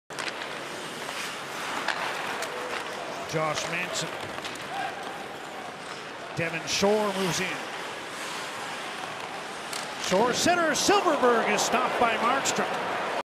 Vancouver Canucks vs Anaheim Ducks | Markstroms save on Silfverberg
Jakob Silfverberg snaps a one-timer from close range, but Jacob Markstrom makes the save with his right pad to keep the Ducks off the board

Never miss daily hockey game highlights of the season